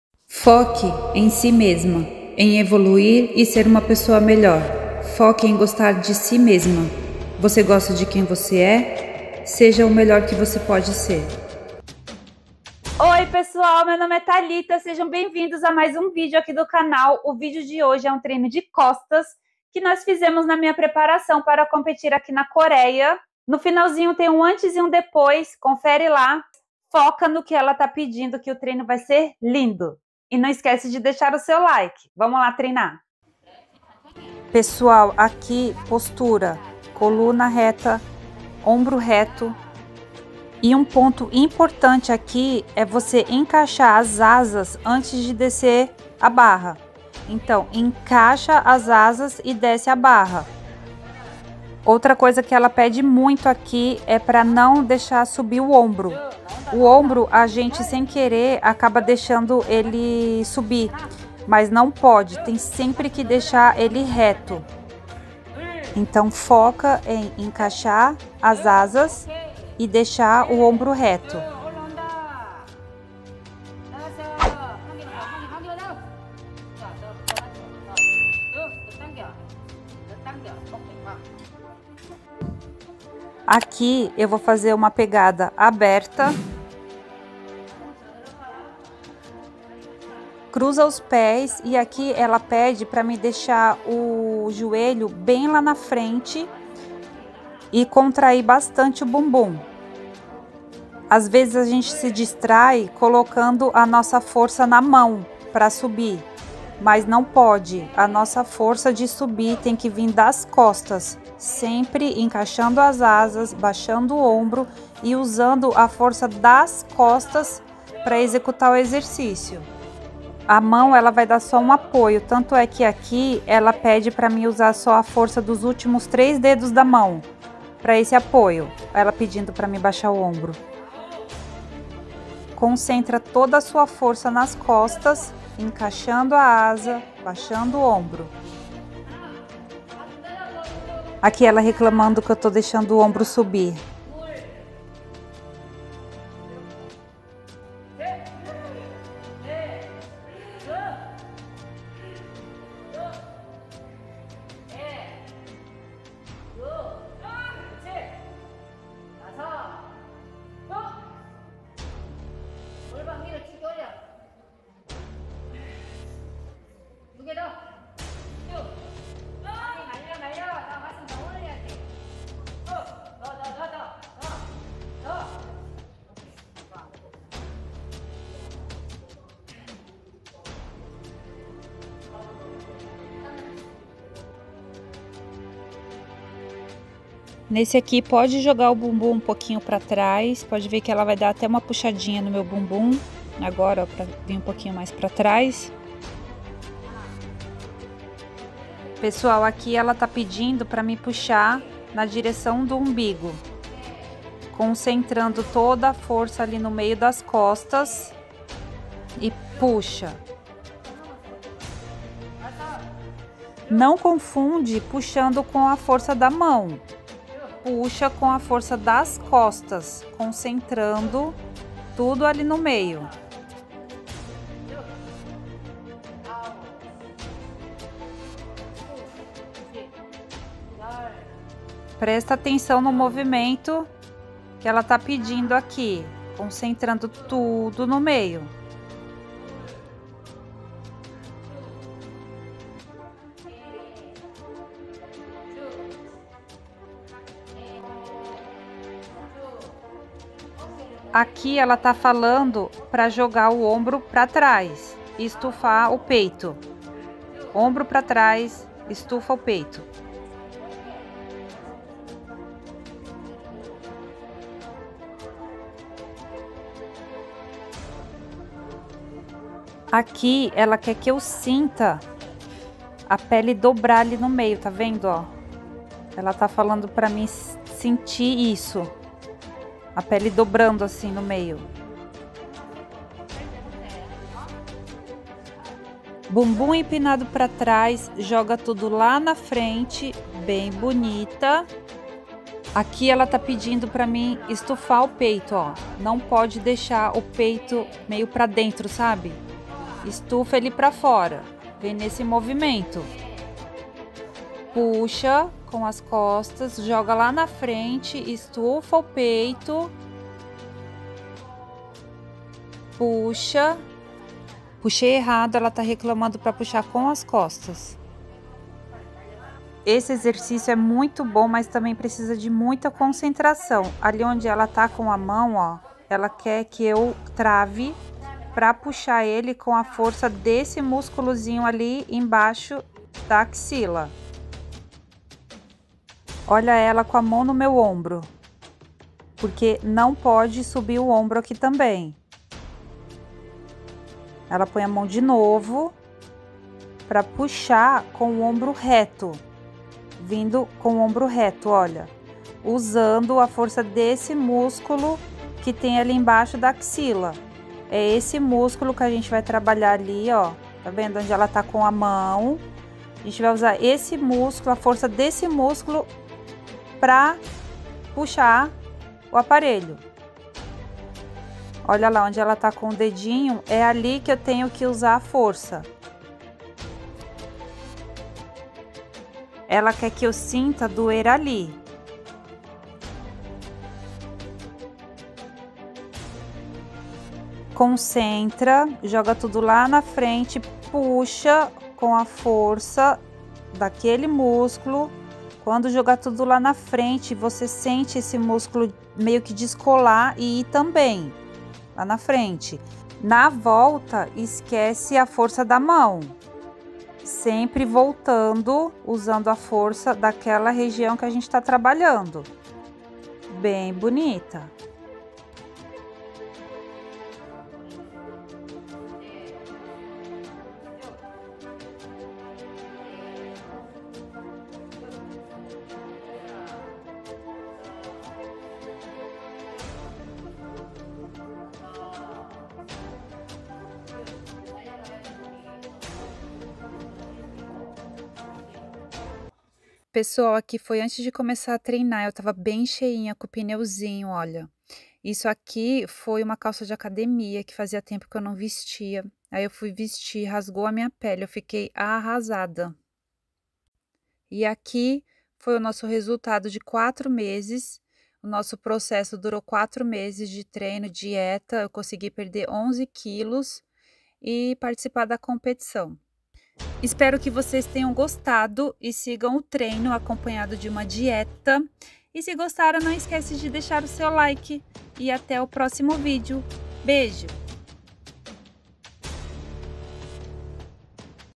TREINO DE COSTAS! COM TREINADORA COREANA. 5 EXERCÍCIOS💪
5 EXERCÍCIOS PARA AS COSTAS. TREINO DE COSTAS PARA AJUDAR NA
DEFINIÇÃO DAS SUAS COSTAS, ESSE TREINO  ME AJUDOU MUITO A DEFINIR MINHAS COSTAS E PODER PARTICIPAR DA COMPETIÇÃO.

THALITA 🙎‍♀️. MÃE DE 3 MENINOS 🙋‍♂️🙋‍♂️🙋‍♂️ VIVENDO NA COREIA DES DE 2007🏚

VEJAM TAMBÉM😃